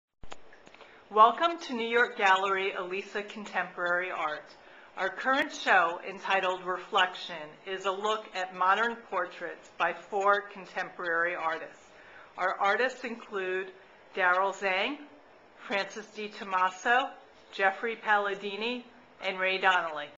New York Art Gallery Elisa Contemporary Art Reflections Exhibi
New York Art Gallery Elisa Contemporary Art presents a video tour of Reflections, a modern look at contemporary portraits.  Reflections featured the artwork of four US artists: Daryl Zang, Francis Di Tommaso, Jeffrey Palladini and Ray Donley.  The exhibit was on display at Elisa Contemporary Art from September 24 - November 21, 2010.